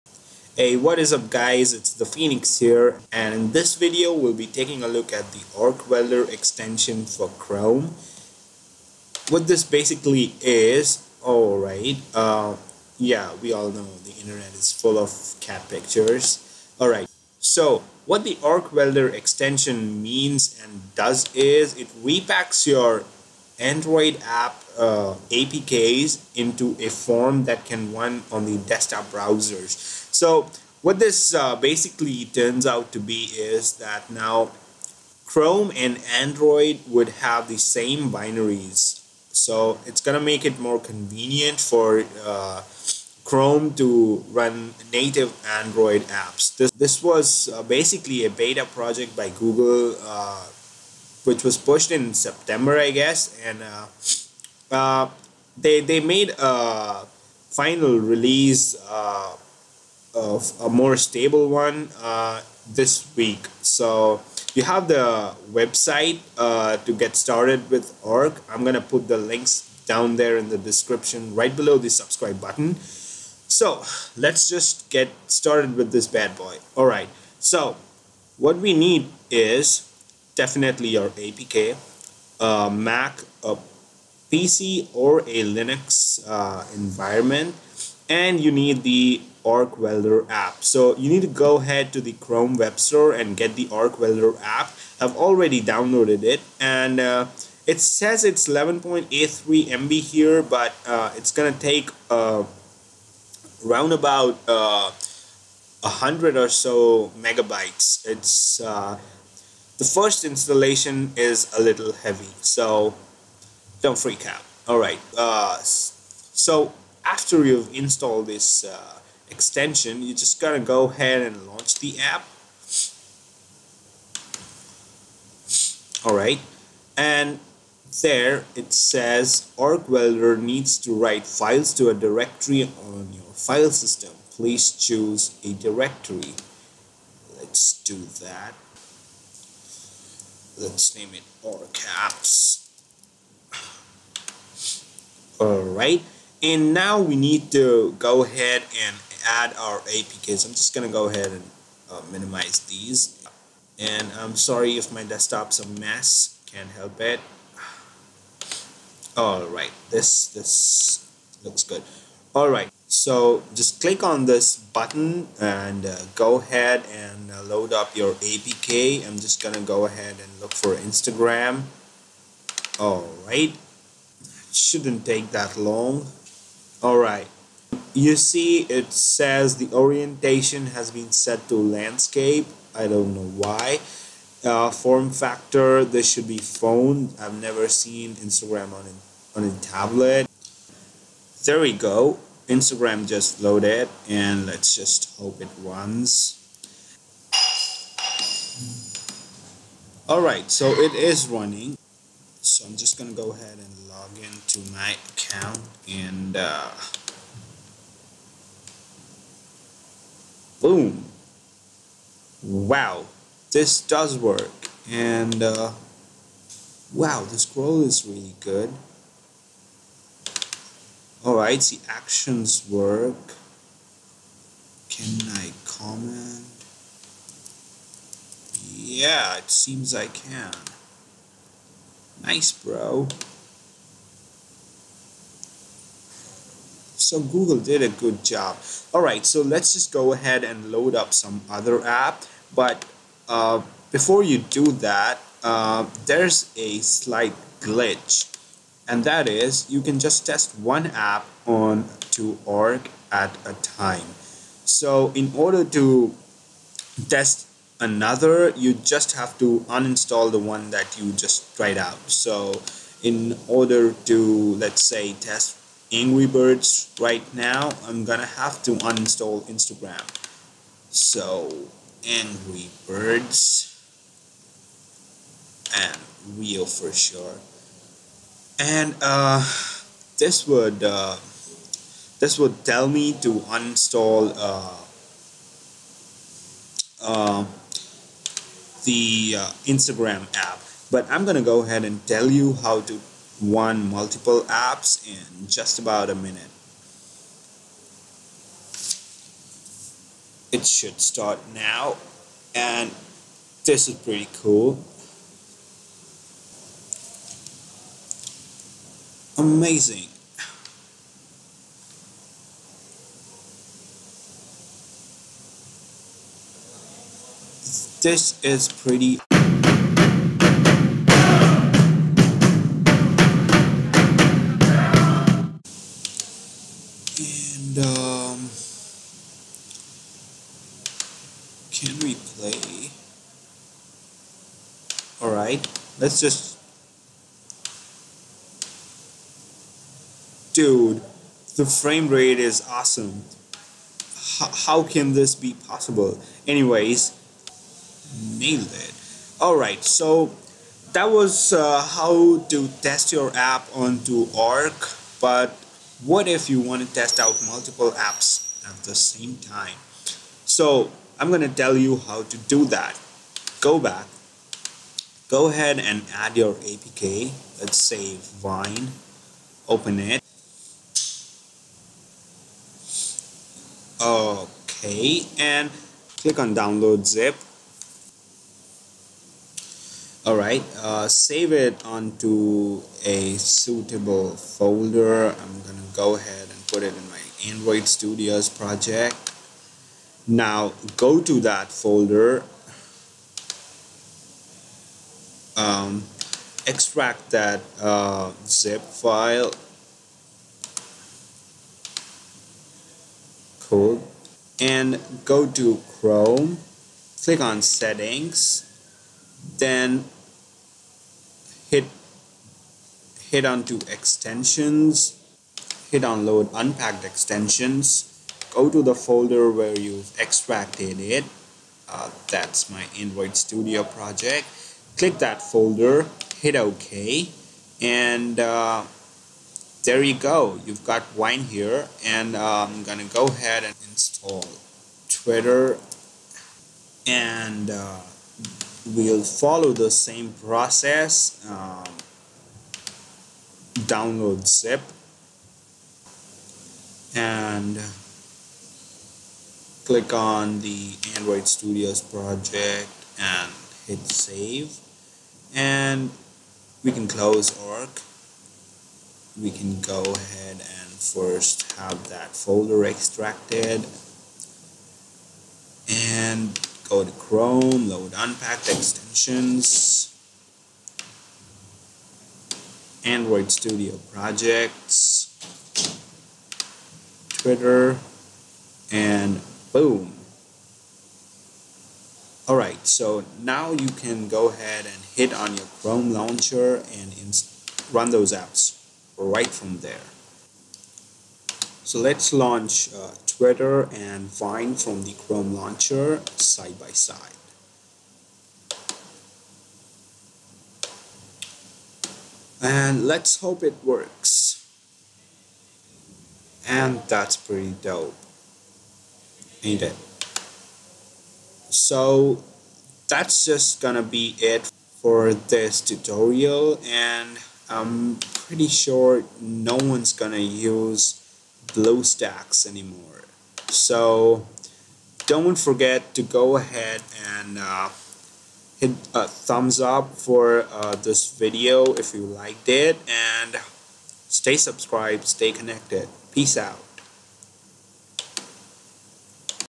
How to Install Android Apps on Pc Without Bluestacks
This video walks you through the process of installing Android Apps and running them natively via Chrome's ARC Welder app which packs the .apk files into a Chrome readable format.

to learn more about ARC and get the ARC Welder app